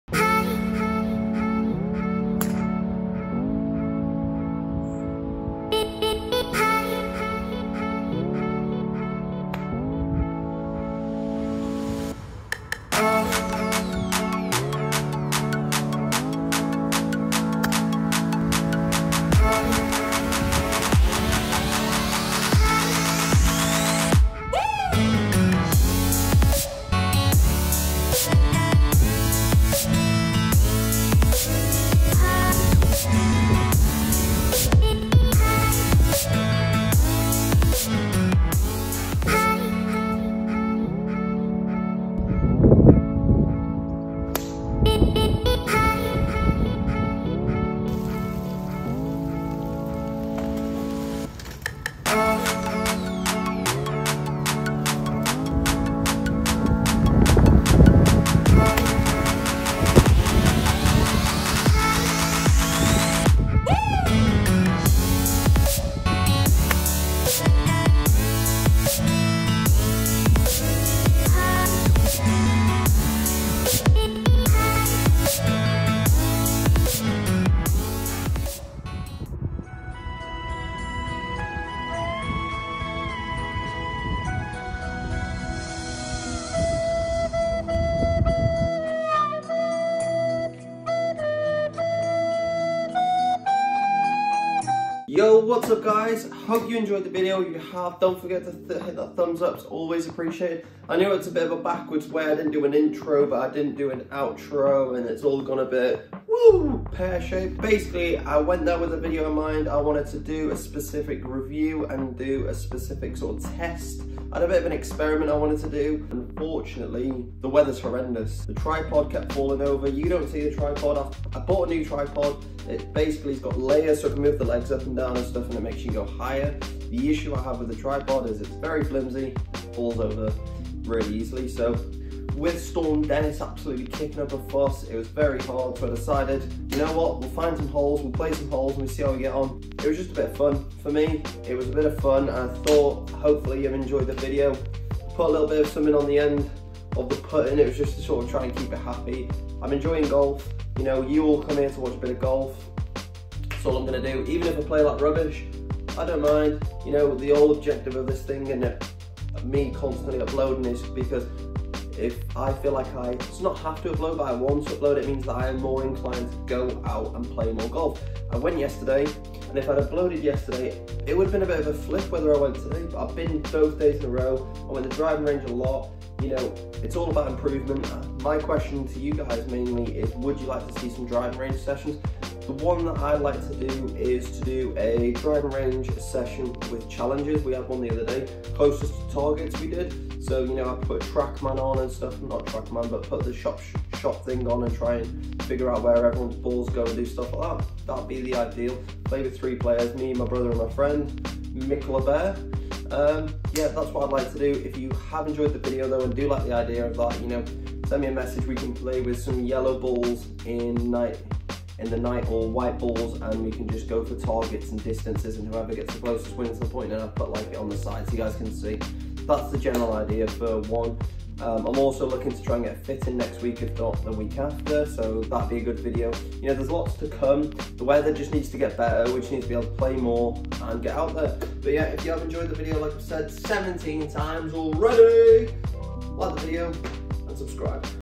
PLAYING GOLF THROUGH STORM DENNIS - BEST PUTT EVER?!
Benjamin Edge shows you him, PLAYING GOLF THROUGH STORM DENNIS - BEST PUTT EVER?!
Hi, I'm Ben a 19 Handicap player who loves golf ⛳, I recently decide to show you all him, PLAYING GOLF THROUGH STORM DENNIS - BEST PUTT EVER?!